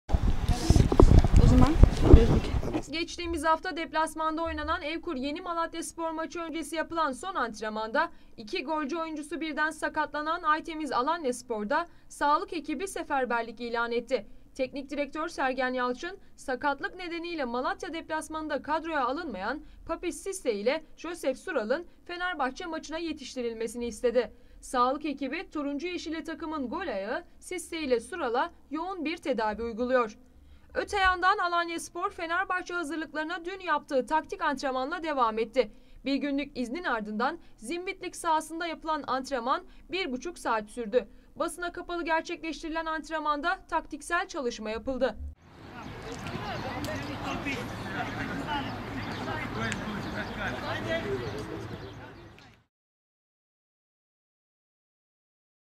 ALANYASPOR'DA FENERBAHÇE ALARMI
Süper Lig’in 29. haftasında oynayacağı Fenerbahçe maçı hazırlıklarını sürdüren Aytemiz Alanyaspor'da golcü oyuncular PapissCisse ve Josef Sural'ınhaftasonuna yetiştirilmesi için yoğun çabasarfediliyor